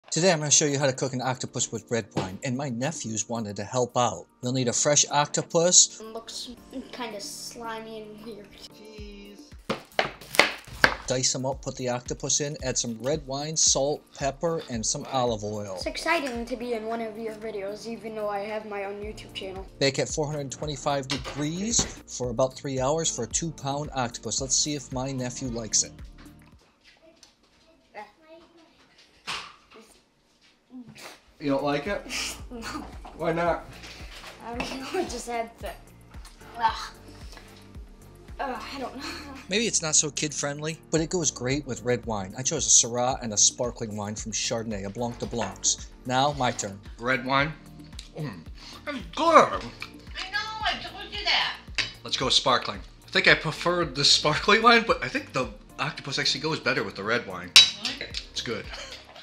How to Cook Octopus with RED WINE | Teaching My Nephews
How to cook Octopus with red wine

Wines featured in this episode:
Piedrasassi, Santa Barbara Country Syrah
Schramsberg, Blanc de Blancs Brut 2018